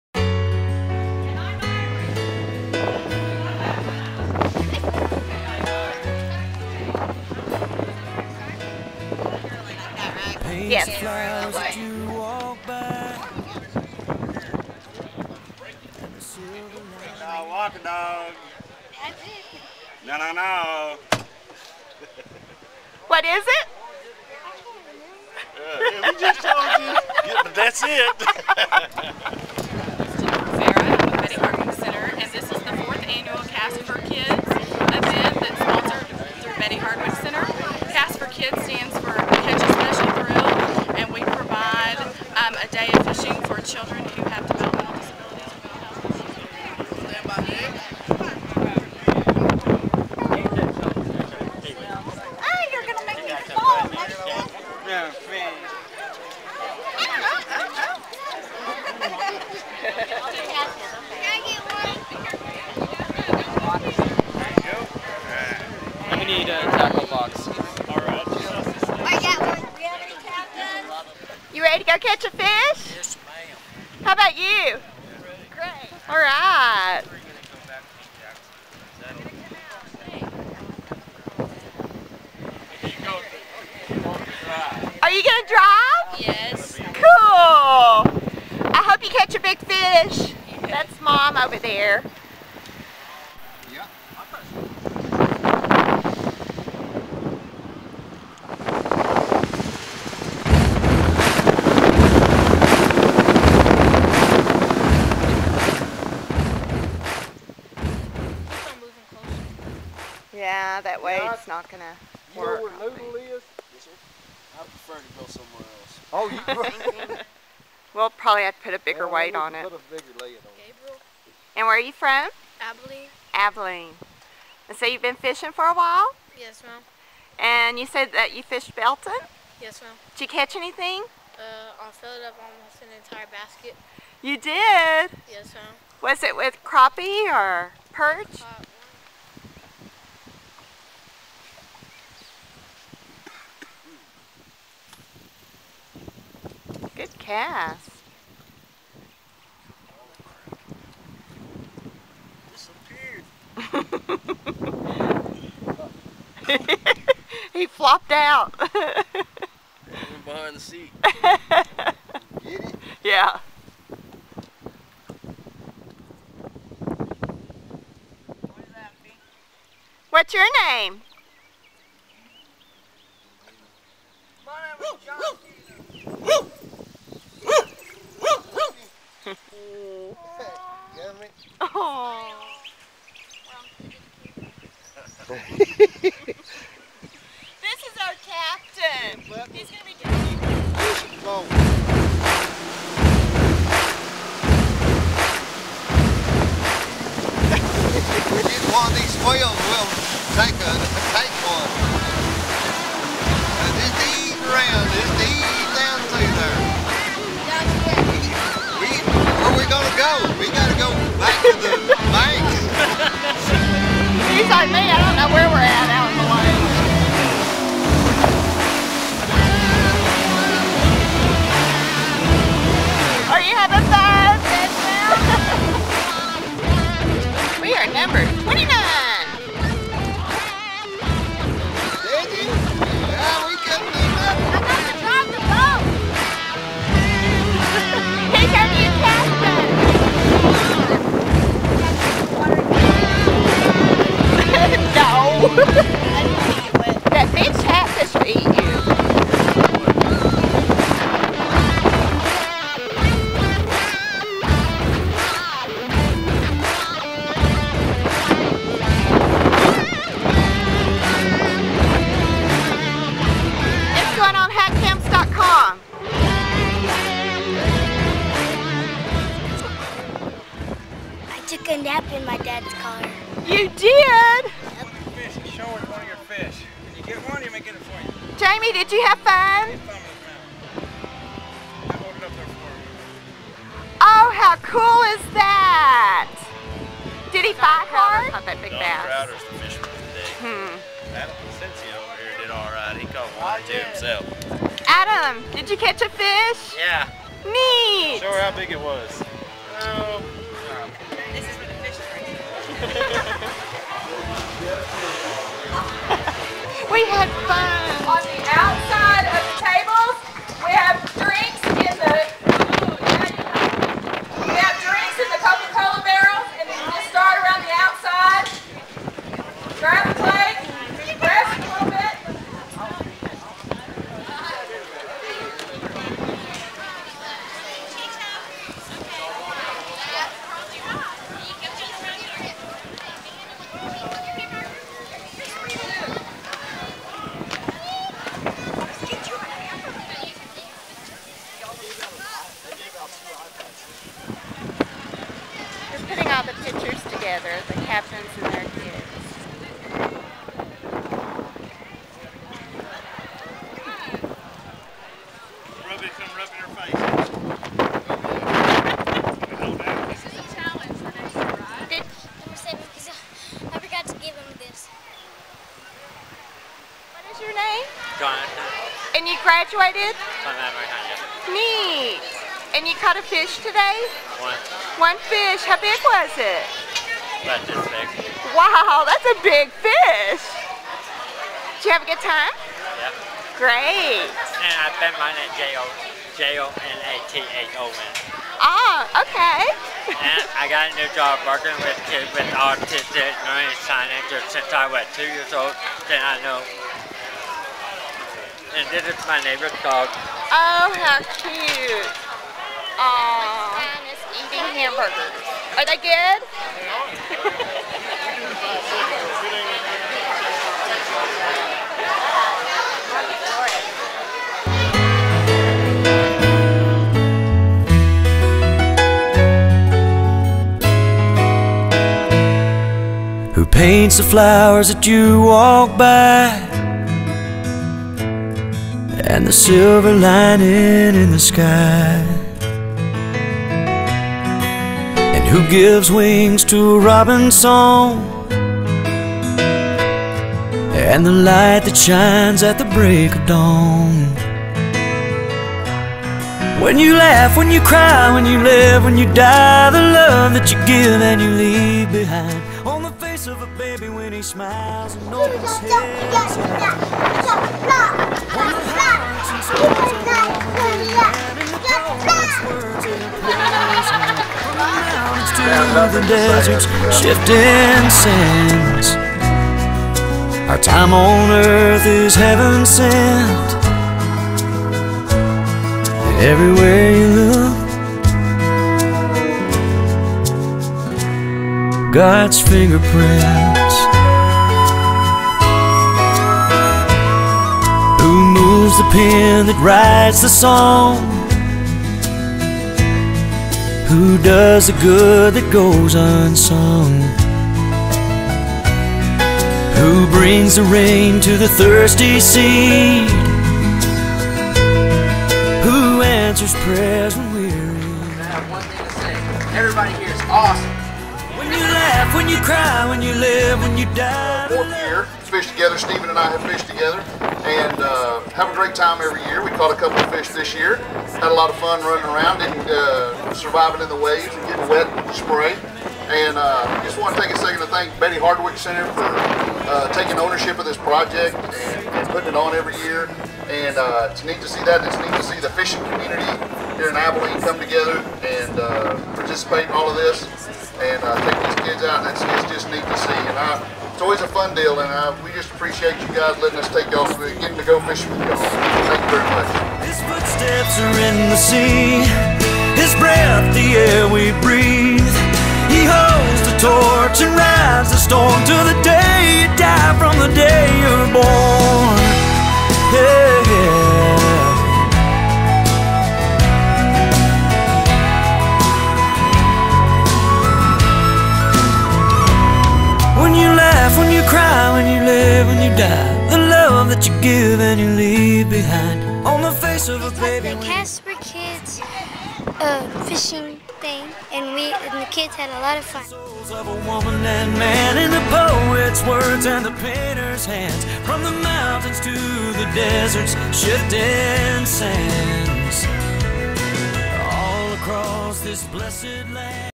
Catch a Special Thrill.....Cast for Kids, through the eyes of Gabriel...
June 12, 2010 Lake Ft. Phantom, Abilene, TX

Early in the morning, we were introduced and paired with Gabriel and his Uncle Domingo. After we got his rod and reel, tackle box, life jacket, worms and minnows in the boat, we said goodbye to his mother, Naomi, & sisters Lexie 14, and Jaidyn 6. Photos, which later were affixed to the award plaques, were taken as we passed by the pavilion.

Gabriel was quiet with little expression as we launched the boat. The wind was high on an overcast day & as we sped away from the dock, you could see apprehension in his eyes. As we cut through the waves, water splashing on us, he seemed to relax & started to smile. It was a wild, fun ride, something he had never experienced before and when we got to the wind protected cove, he was anxious to start fishing.

Quietly and without emotion, he reached for his rod and reel and after baiting it with a worm, made a one-handed cast out toward the lake. I could tell he had fished before, so I took this opportunity to talk with him. As he blankly stared at his line, he slowly revealed that he had been fishing before and loved it. As I kept talking and trying to get him to open up to me, I could see a slight gleam come into his eyes, and then a huge smile. he told me about his trip to Lake Belton and how he filled a basket with fish, all kinds of fish and of his love of swimming.

Even though he had a fish on but lost it, we could tell he was getting discouraged. We asked him if he would like to drive the boat and be our captain. Without hesitation, he jumped into the captain's seat, still emotionless, with Dewey at his side. After a short explanation of how to operate the boat, Gabriel fired up the engine, put it into gear, stepped on the hotfoot and off we went.

Gabriel was the captain, the man in charge, negotiating and maintaining control as we went through the rough waters on our return to the dock. As tension turned into relaxation, you could see he was having the time of his life. The expression on his face went from a blank stare to a smile and then unleashed to a big grin and then to laughter! He was a free spirit!

At the dock, Gabriel held our number up so they could get our truck and we could load the boat. He could not wait to tell everyone how he had drove the boat and he was captain!

After a picnic lunch including hamburgers, chips, cookies, and drinks, it was time for the awards ceremony. With an overwhelming sense of pride and compassion, one by one the captains presented their young boaters with a plaque and picture. It was finally Gabriel's turn. He became center stage of the show and after presenting his award to him, we all walked away with a new outlook on life.

Thank you Gabriel for changing our lives.

Team Nichols, Dewey & Carolyn
Gabriel is 11, will be 12 in August, and was born in Abilene, Tx, He will be in the 6th grade this year, favorite class being math. He loves football, Toni Romo, Marion Barber and the Dallas Cowboys. He has been diagnosed with Bipolar disorder, ADHD and ODD, as well as asthma. He struggles with his anger, but is learning to handle it. Gabriel's mother, Naomi, states "Gabriel is a very good young man, but as his mother, it is hard to handle him at times but we deal with it as we go. Sometimes his Dad & I feel like we need a break cause it is so hard dealing with Gabriel... His older sister Lexie can talk him into calming down. I have to say that he loves his sisters very much, even though they pick and argue with each other!!!"

In a note later from Naomi,
     Gabriel says, "Cast for Kids was FUN and I want to do it again. It felt good to be with Carolyn and Dewey out on the boat cause usually people don't like to be around me cause I don't know how to behave."

I was so happy and thankful to everyone who showed up for Cast for kids!! We want to THANK YOU and YOUR HUSBAND for all that you did that day!! Gabriel and his uncle had a lot of fun!!! We are glad to have met you and your Husband!!! I'm glad Gabriel got to boat with You both!!!
Thank you, Naomi